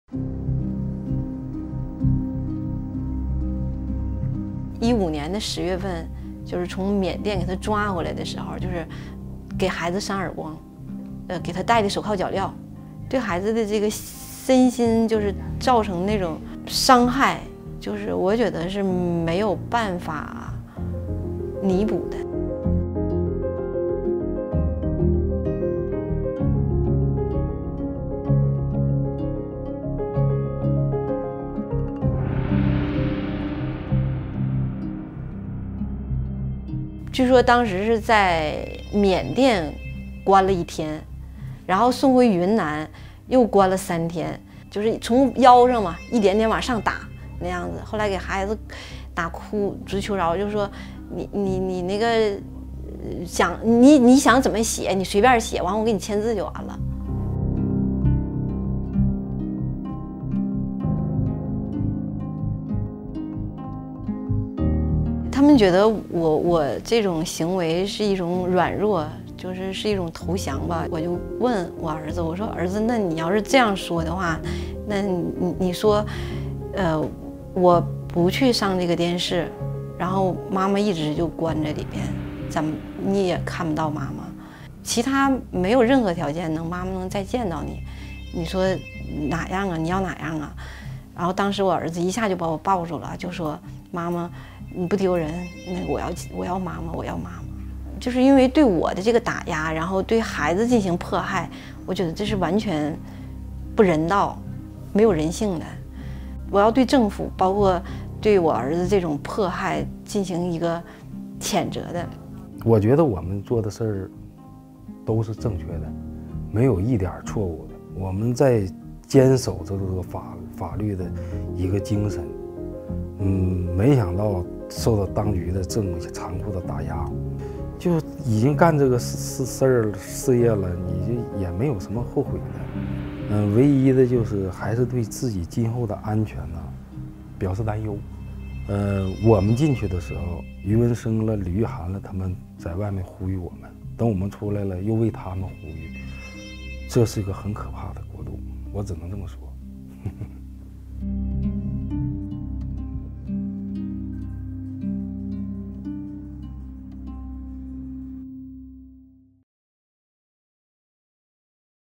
China's 709 crackdown lawyer Wang Yu condemns authorities for the torment of her son
The Chinese government initiated a crackdown against human rights lawyers in China on July 9, 2015 to tighten their grip on the legal profession.

Wang Yu, the first to be arrested during this wave of detentions, condemns Chinese authorities for the torment of her son.